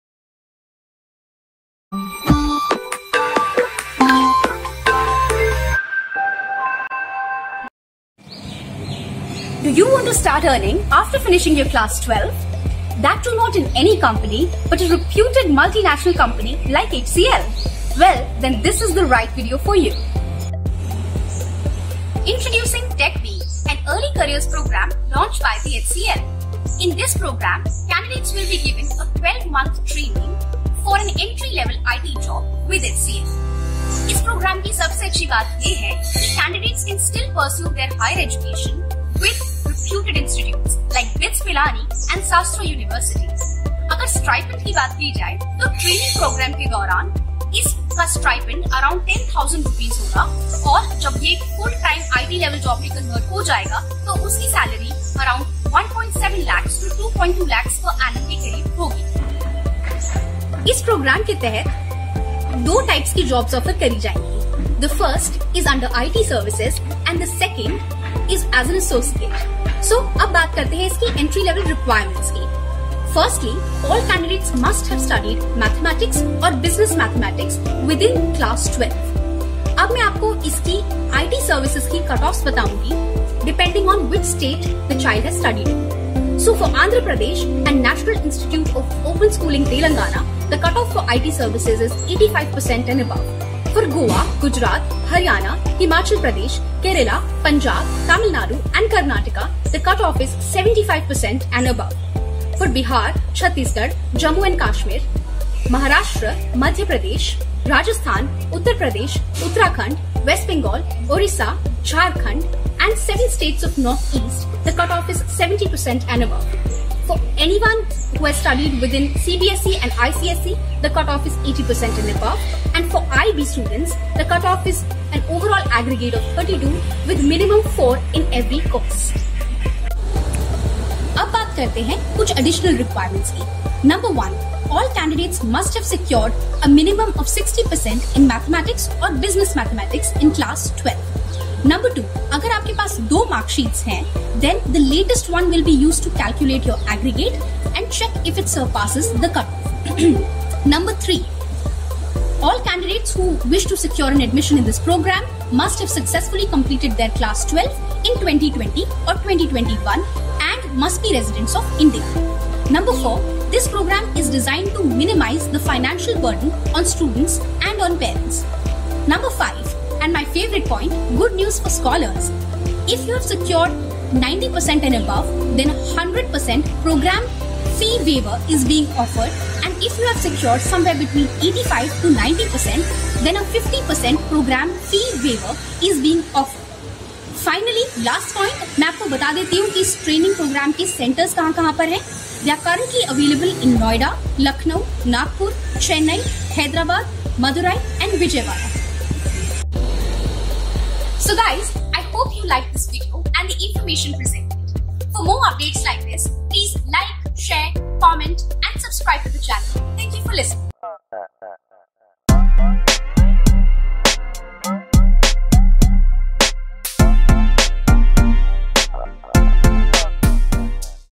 HCL TechBee 2021. Best IT Career Option After 12th Class
TechBee is a great career choice for those class 12th students who want to start earning salary right after their schools. It also gives opportunity to peruse your higher education from top universities while working with HCL Technologies, a reputed IT company in India.

HCL TechBee 2021 Registration Open. Watch this video till end to Fill Application Form Online